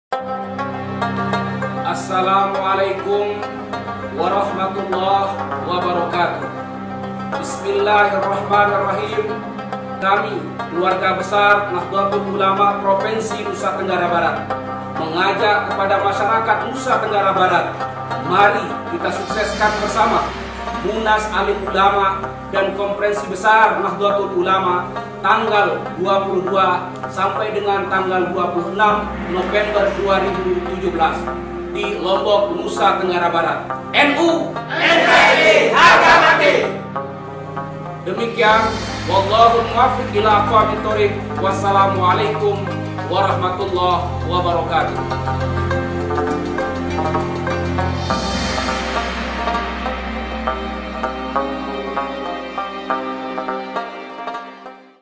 MUNAS ALIM ULAMA & KONFERENSI BESAR NAHDLATUL ULAMA
MUSYAWARAH NASIONAL ALIM ULAMA DAN KONFERENSI BESAR NAHDLATUL ULAMA

Memperkokoh Nilai Kebangsaan Melalu Gerakan Deradikalisasi dan Penguatan Ekonomi Warga